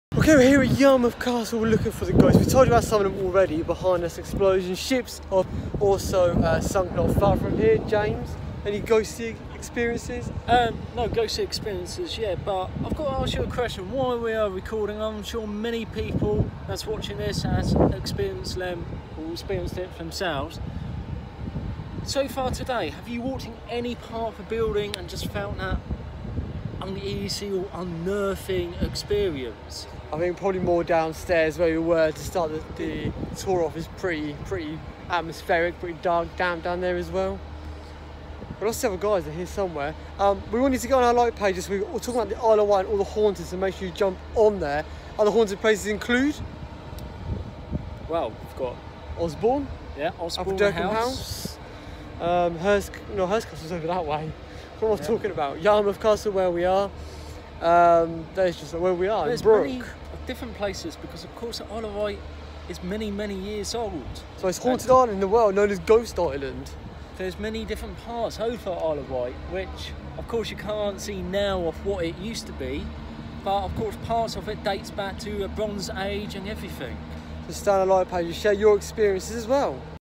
IOW- Why is is known at Ghost Island
Andrew and james reflect on the most haunted island